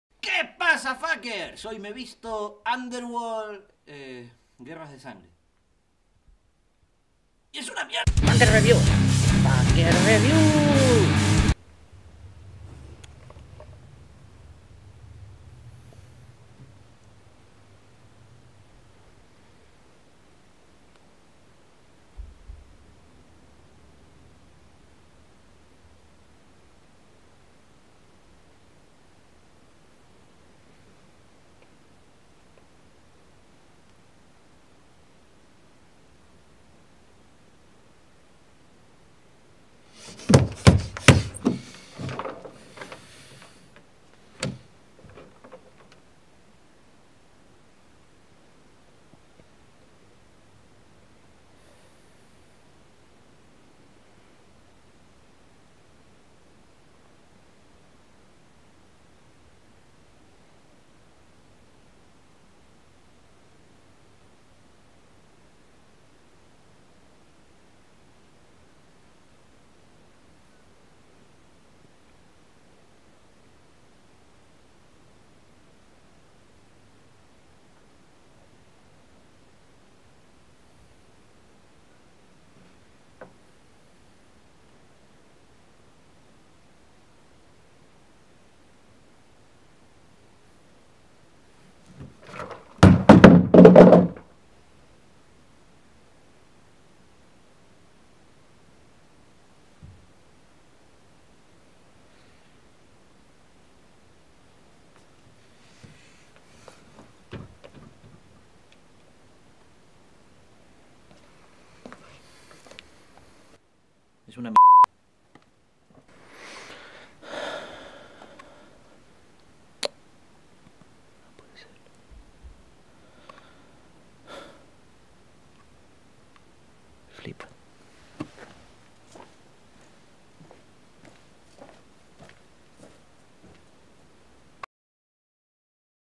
Faquer Review | Underworld: Guerras de sangre
Review demasiado faquer para esta cosa que debió quedarse en el inframundo...